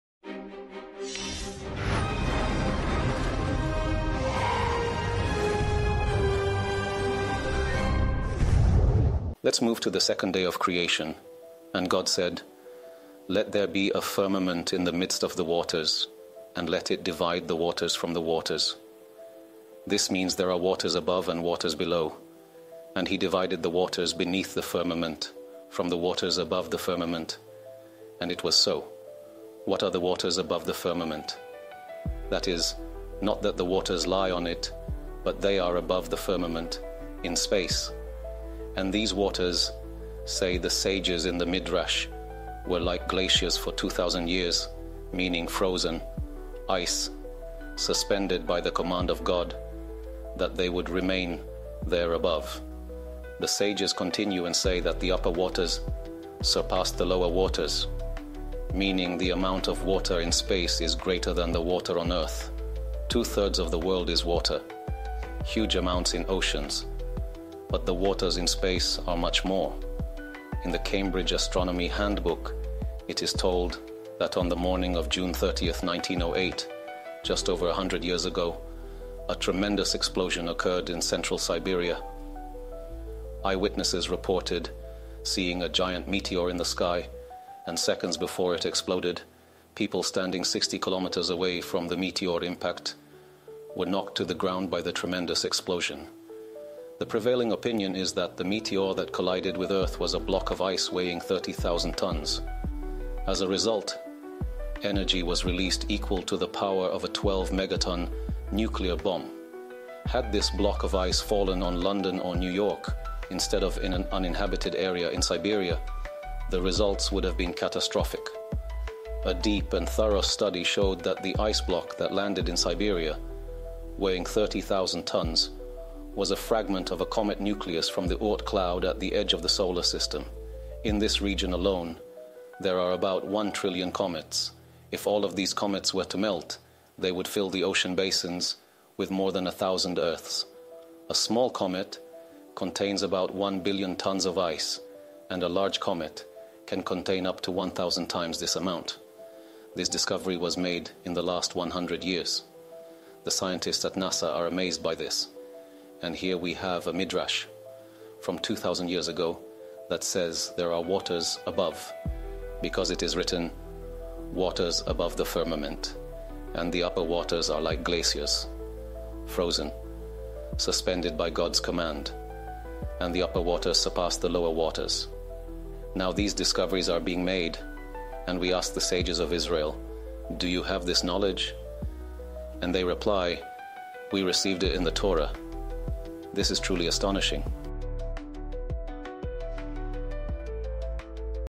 The Age of the Universe – Water in Space (AI)
Dubbed using artificial intelligence

In this fascinating lecture, Rabbi Zamir Cohen explores the Torah's description of "waters above the firmament" in light of modern scientific discoveries. Ancient Jewish sages described vast amounts of frozen water suspended in space over 2,000 years ago—quantities far exceeding Earth's oceans.

Did the Torah precede modern science? How do these ancient teachings align with NASA’s findings on comets and massive ice bodies in space?

Join this intriguing journey into the remarkable harmony between Torah wisdom and astronomy.